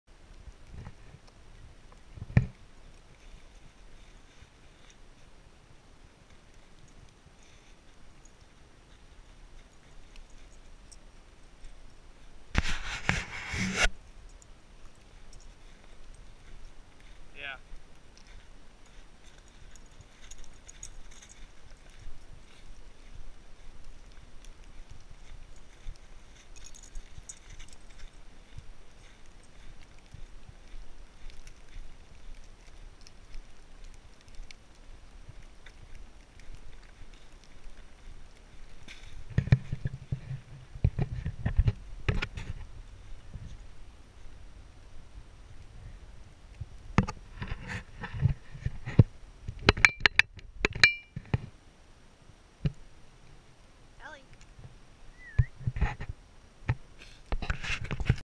Nitecore TM26 test on snowy night
A little Video to show how bright the Nitecore TM26 is